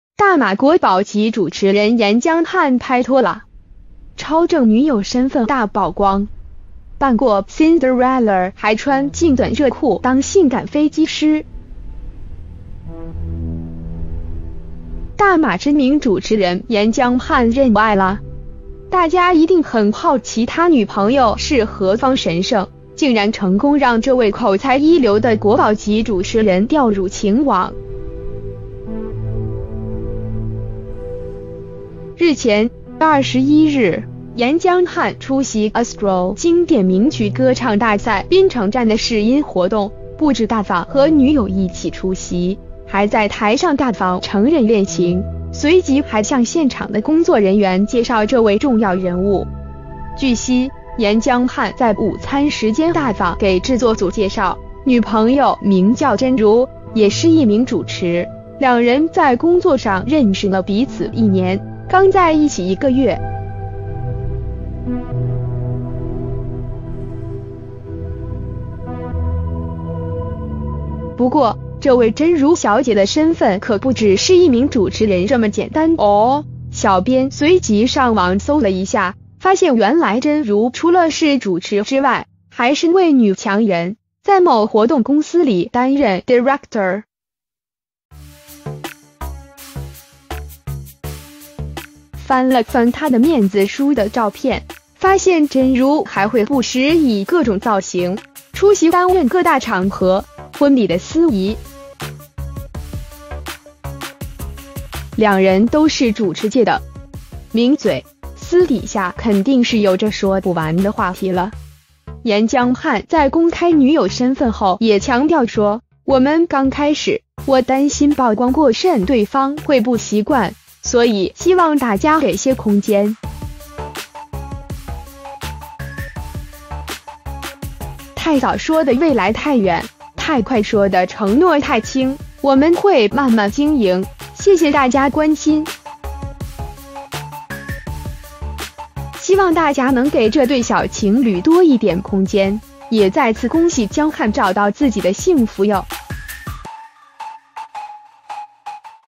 大马国宝级主持人颜江翰拍拖啦❤ 超正女友身份大曝光！扮过Cinderella还穿劲短热裤当性感飞机师?
大马知名主持人「颜江翰」认爱啦～ 大家一定很好奇他女朋友是何方神圣，竟然成功让这位口才一流的国宝级主持人掉入情网！

日前（21日）颜江翰出席《Astro经典名曲歌唱大赛》槟城站的试音活动，不止大方和女友一起出席，还在台上大方承认恋情，随即还向现场的工作人员介绍这位重要人物！据悉，颜江瀚在午餐时间大方给制作组介绍，女朋友名叫桢茹，也是一名主持，两人在工作上认识了彼此一年，刚在一起一个月。不过，这位桢茹小姐的身份可不只是一名主持人这么简单哦~小编随即上网搜了一下，发现原来桢茹除了是主持之外，还是位女强人，在某活动公司里担任Director。翻了翻她的面子书的照片，发现桢茹还会不时以各种造型，出席担任各大场合、婚礼的司仪。两人都是主持界的 “名嘴”，私底下肯定是有着说不完的话题了！颜江翰在公开女友身份后也强调说，“我们刚开始，我担心曝光过甚对方会不习惯，所以希望大家给些空间。太早说的未来太远，太快说的承诺太轻，我们会慢慢经营，谢谢大家关心。” 希望大家能给这对小情侣多一点空间，也再次恭喜江瀚找到自己的幸福哟~！